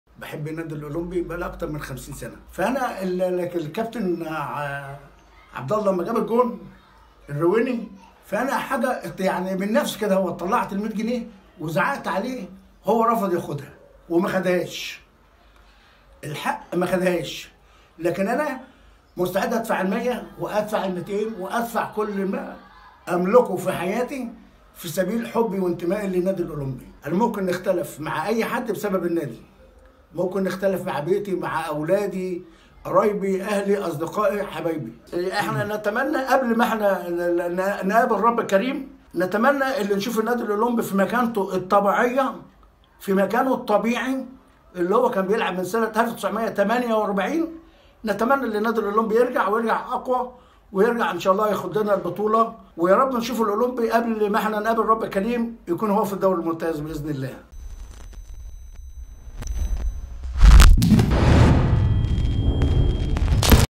في الدرجة الثانية - قصة 100 جنيه هدية من مشجع الأوليمبي لمهاجمه بعد تسجيل هدف
عبد الله الرويني لاعب الأوليمبي سجل هدفا في شباك فاركو بدوري الدرجة الثانية
ليقوم أحد مشجعي الأوليمبي في المدرجات بإخراج 100 جنيه كمكافأة للاعب على الهدف

في الجول تواصل مع المشجع ليحكي سبب قيامه بذلك

💬"مستعد أدفع كل ما أملكه في سبيل حبي وانتمائي لنادي الأوليمبي.. وأتمنى رؤية الفريق في مكانته الطبيعية"